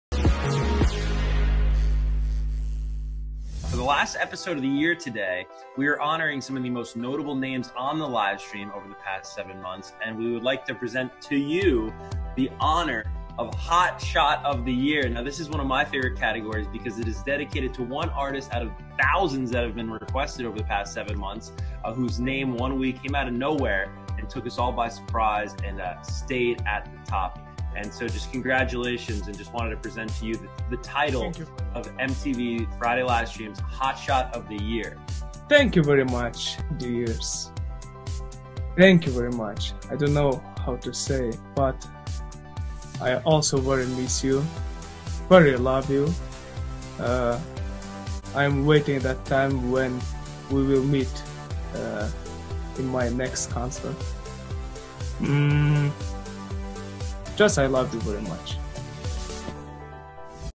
DIMAS RECONOCIDO POR MTV COMO UNO DE LOS ARTISTAS MÁS INFLUYENTES DEL 2020
Dimash reconocido por MTV como uno de los Artistas más influyentes del 2020.
Felicitaciones Димаш Құдайберген!!

👍El día de hoy Dimash obtuvo el segundo puesto en el TOP 5 de los más solicitados, en ésta semana de fridayLivestream 🎉❤️

Además fué reconocido como uno de los artistas más influyentes en MTV de éste año 😃💪💕

MARAVILLOSO TRABAJO DEAR!!

Cr. subtitulos y traducción @claudia_juhn Equipo DKPerú World

* Todos los materiales en el video se utilizan para promocionar el trabajo de Dimash Kudaibergen, Los COPYRIGHT son de sus propios dueños